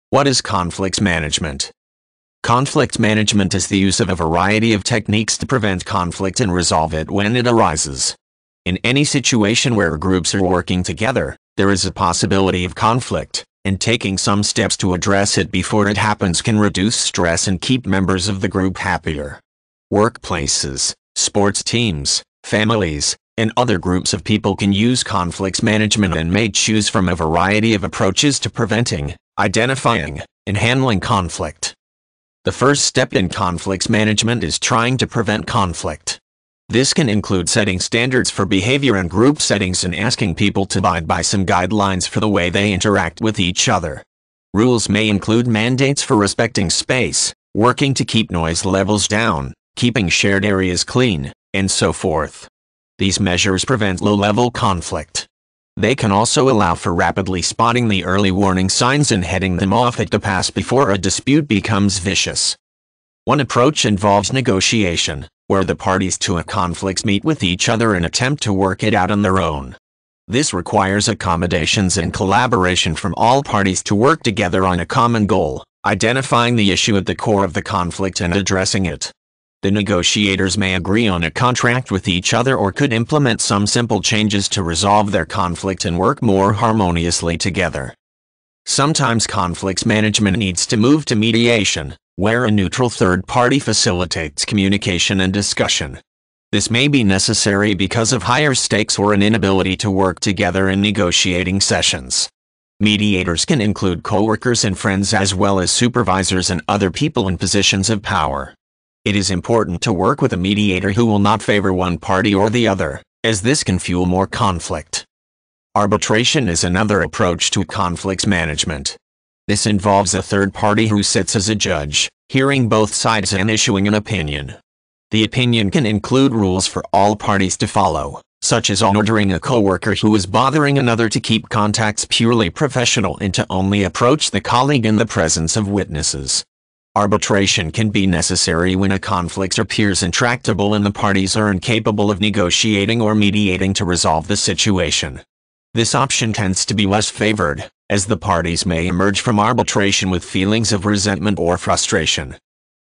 What is Conflict Management
Conflict management is the use of a variety of techniques to prevent conflict and resolve it when it arises. In any situation where groups are working together, there is a possibility of conflict, and taking some steps to address it before it happens can reduce stress and keep members of the group happier. Workplaces, sports teams, families, and other groups of people can use conflict management and may choose from a variety of approaches to preventing, identifying, and handling conflict.